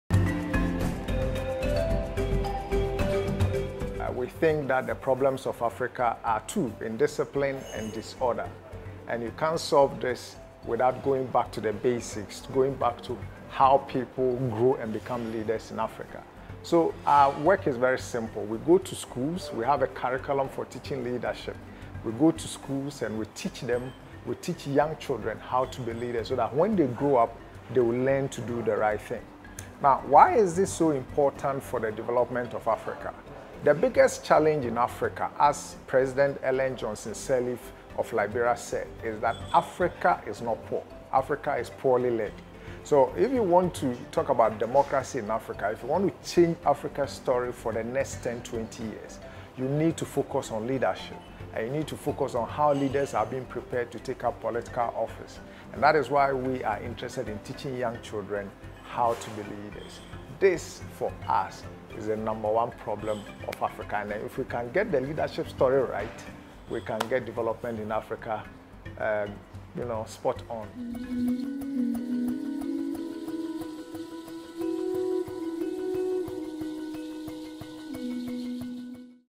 African Youth Activist From Ghana Discusses the Importance of Political Participation
"If you want to change Africa's story for the next 10, 20 years, you need to focus on leadership, and you need to focus on how young political leaders are prepared for office." - Michael Ohene-Effah of LeadAfrique International